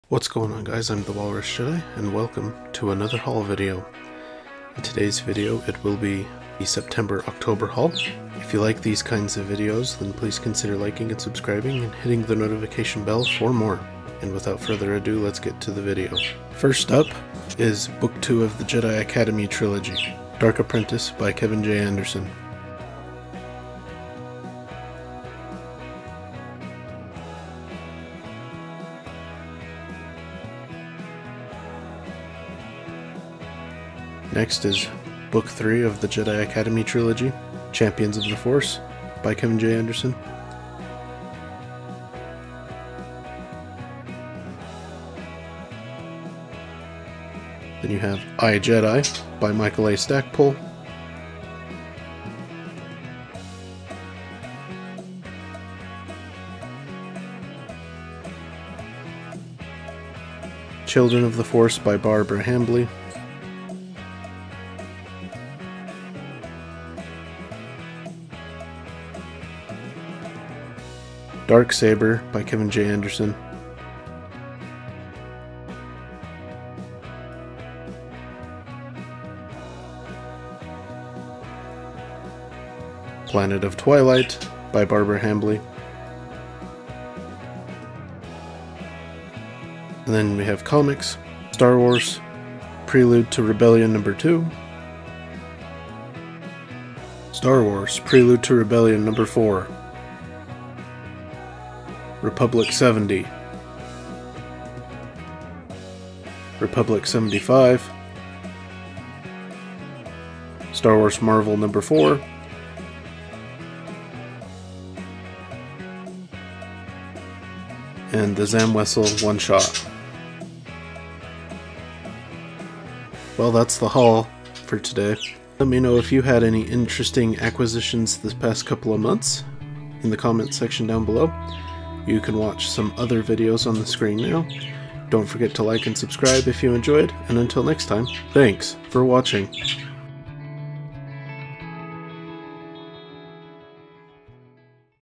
September/October 2024 Haul
I'm the Walrus Jedi and in this video I'll be showing some cool books and some other stuff to expand my collection.